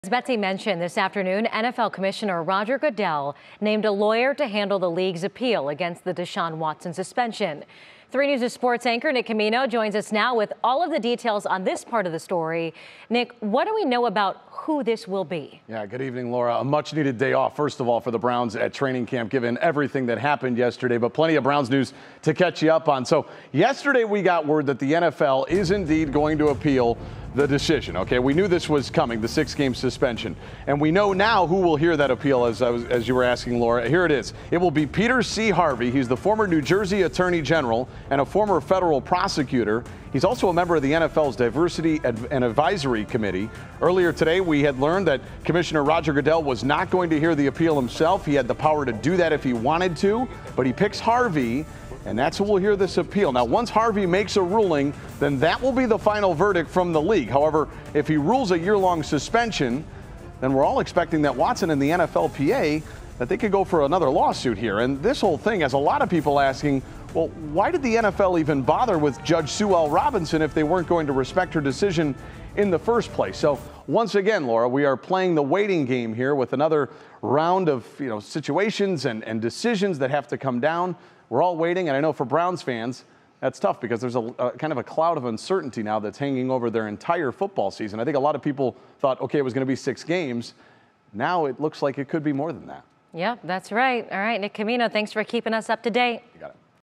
NFL appoints Peter C. Harvey to hear Deshaun Watson appeal
On Thursday, NFL Commissioner Roger Goodell announced that lawyer Peter C. Harvey will serve as his designee in the league's appeal of Cleveland Browns quarterback Deshaun Watson's six-game suspension.

Notably, Harvey is a member of the NFL's Diversity Advisory Committee and served as an adviser to the league during its personal conduct investigation into Dallas Cowboys running back Ezekiel Elliott in 2017.